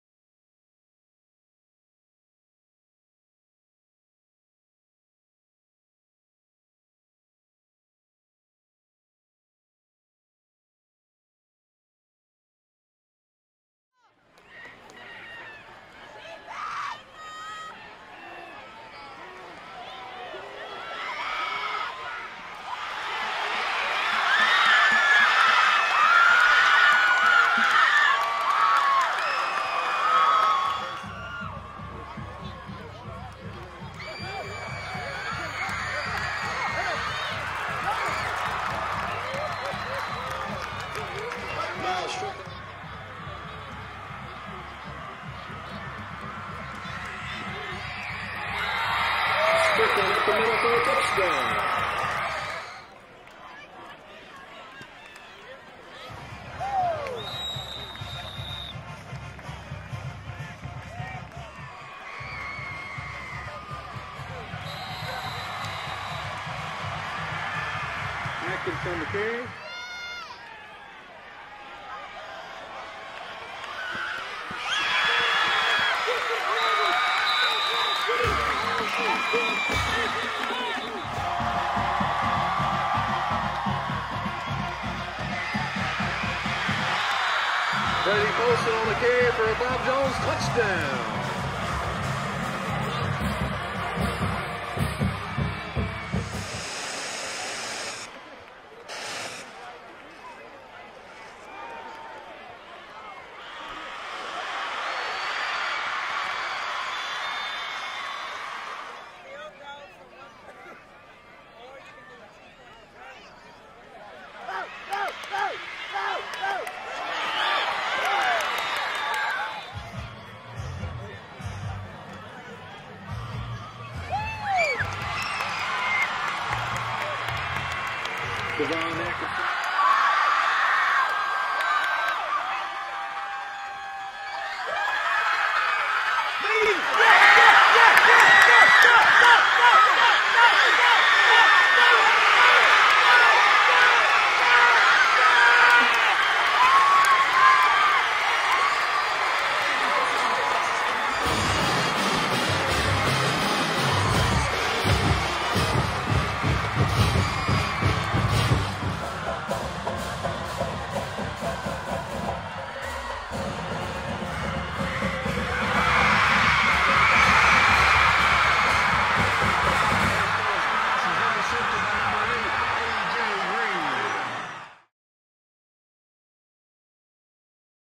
AHSAA FOOTBALL: BOB JONES 27, JAMES CLEMENS 24 9.4.2015
MADISON, ALABAMA -- For the second straight week, Bob Jones has come from behind to get the win. Miles Stricklands 98 yard catch and run gave the Patriots the 27-24 win over James Clemens in the Battle of the Madison Bowl.  (Robert Ballard/rocketcit